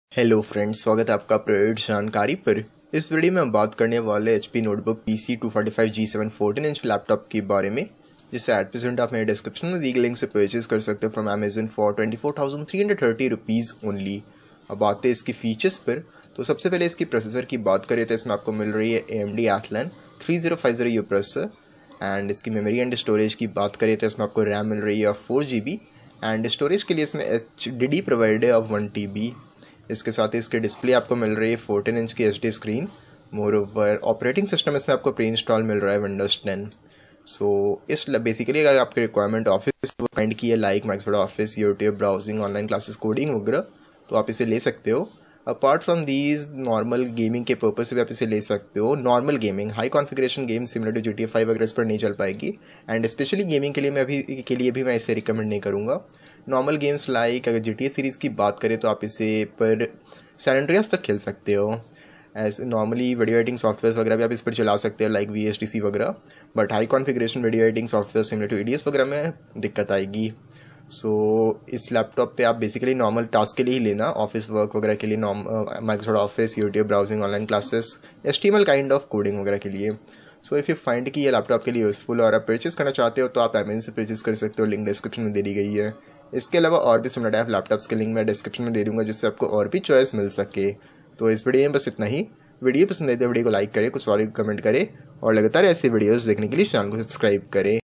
HP Notebook PC 245 G7 14" (35.56cms) Laptop ATHLON-3050U/4GB/1TB/Windows 10 (21Z04PA) reviews
Purchase from Amazon

This video gives you a brief detail and review of HP Notebook PC 245 G7 14" (35.56cms) Laptop ATHLON-3050U/4GB/1TB/Windows 10 Home/ 1 Year Warranty (21Z04PA).